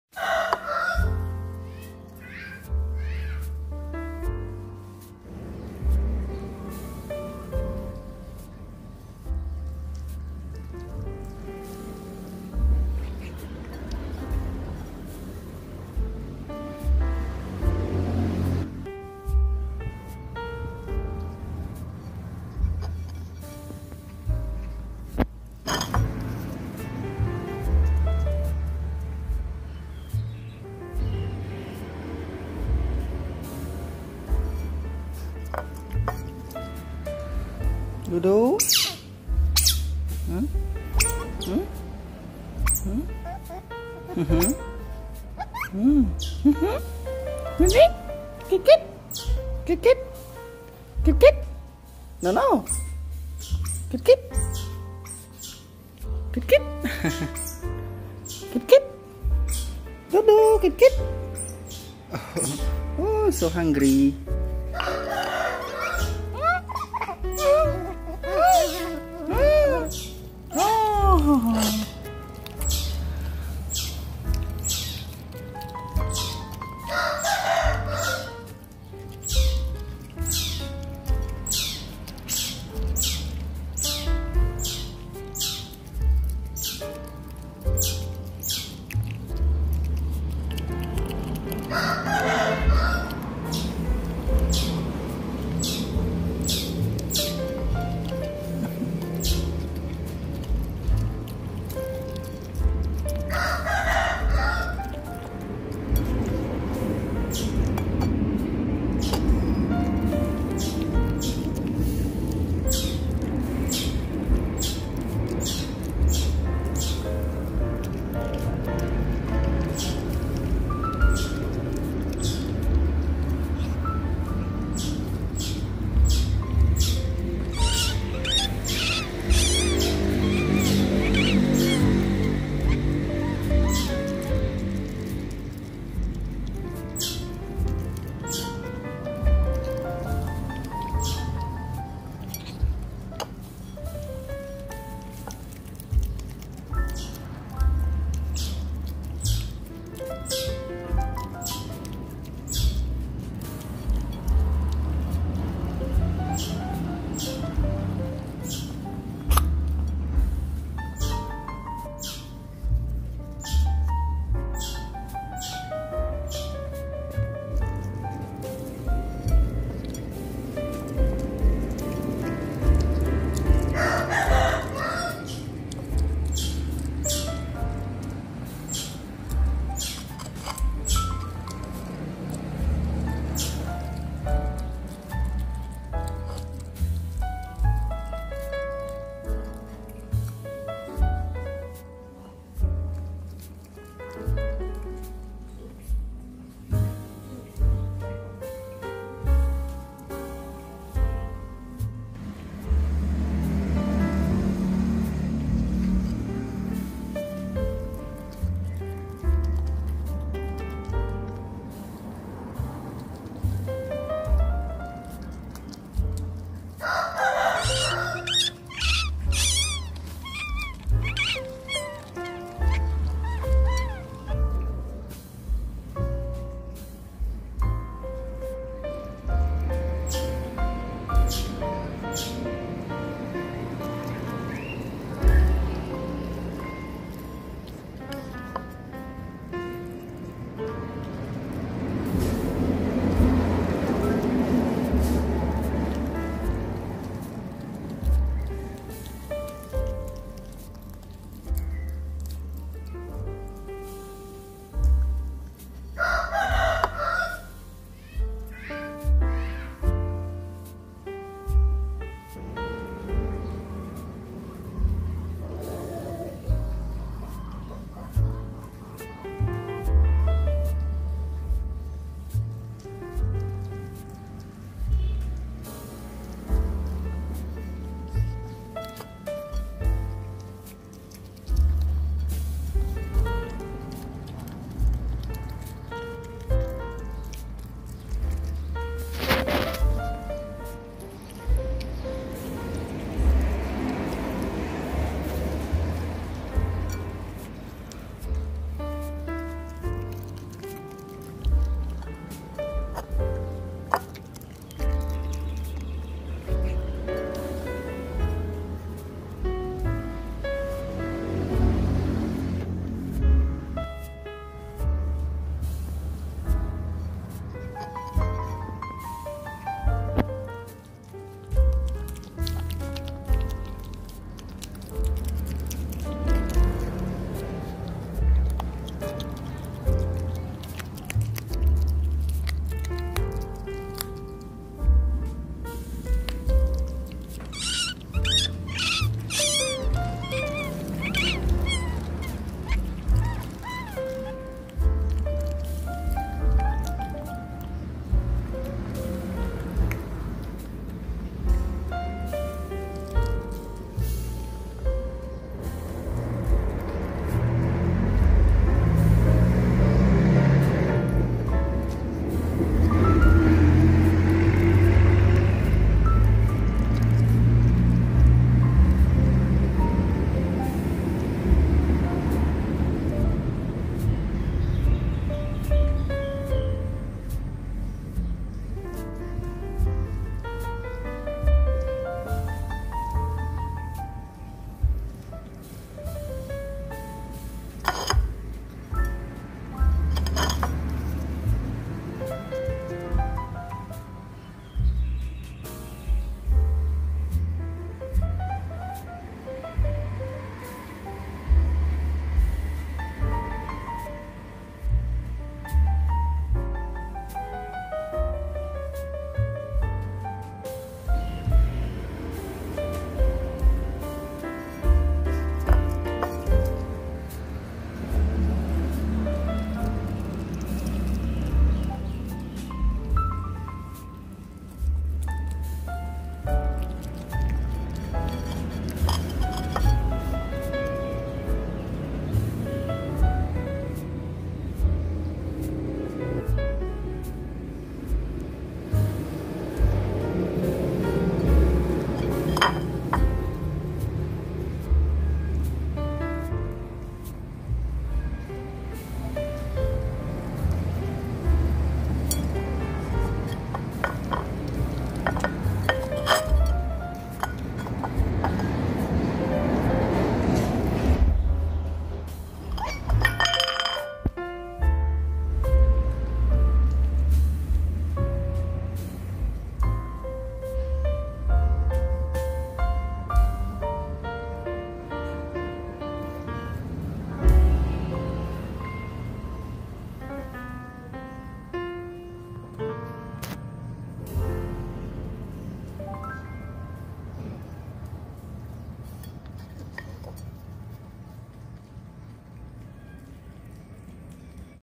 DoDo yummy yummy fish so funny 😂😍😘🥰💖💝🦦🐟♥️♥️♥️
Nika and DoDo at a young age have to spend a lot of milk every day. I'm poor I hope you understand me. They are lovely, I love them like my children in my family. They were adopted by me because a hunter hunted their mother, so I took them. 😭😭😢🦦🦦🍼🍼🍼🍼🍼🍼💕💕🥰🥰
Please 🙏🙏